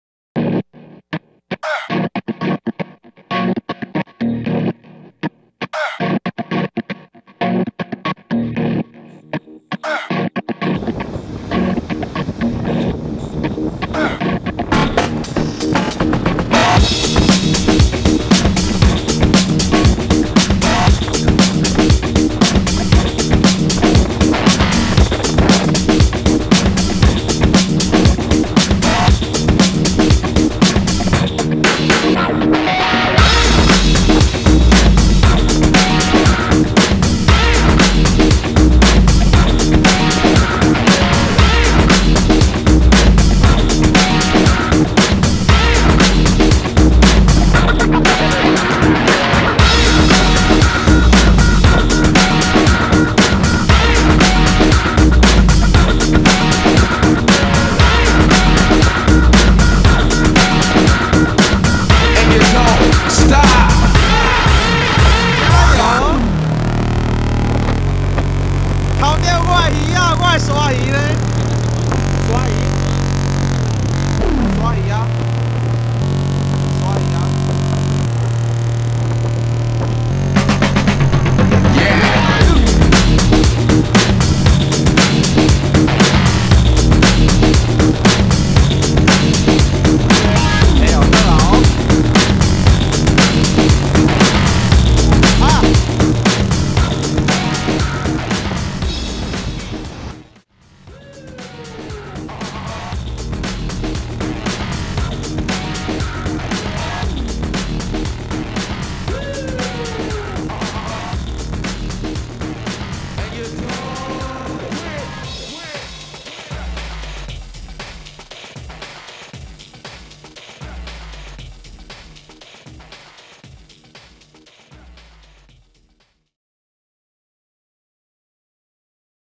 2013.10.18 半島傳說x海生演釣研-豬仔- 岸拋鬥鯊 ( shore popping；撃投-ショア磯)
眼見沙魚飛躍水面炸裂四次，
smith 的 77IIB ROCKET大凹！
無奈，就快將魚兒放倒之際，
Owner 250lb路亞環竟開掉！
但光看鯊魚水表炸裂的熱血沸騰瞬間
絕對是非常難得且難忘的回憶